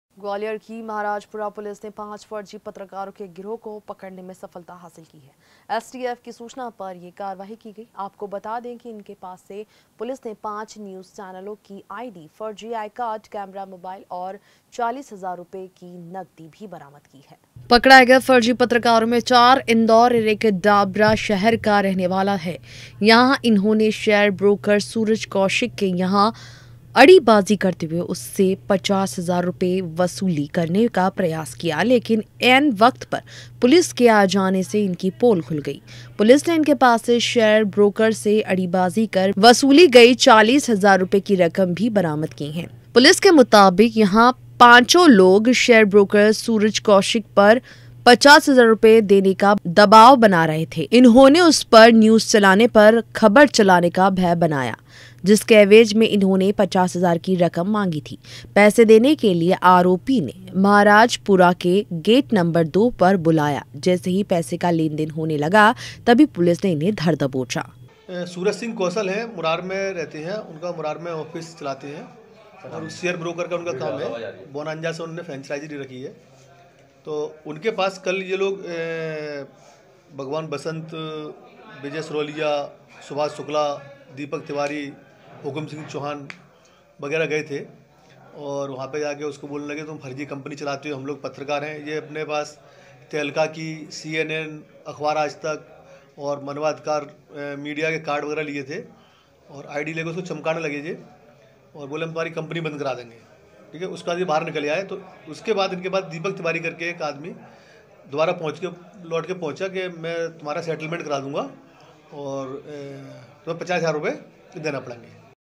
GWALIOR 5 फर्जी पत्रकारों का गिरोह
ग्वालियर की महाराजपुरा पुलिस ने 5 फर्जी पत्रकारों के गिरोह को पकड़ने में सफलता हासिल की हैं.. एसटीएफ की सूचना पर यह कर्यवाई की गई हैं...आपको बता दें कि इनके पास से पुलिस ने 5 न्यूज चैनलों की आईडी... फर्जी आईकार्ड...कैमरा, मोबाइल और 40 हजार रूपये की नकदी भी बरामद की हैं...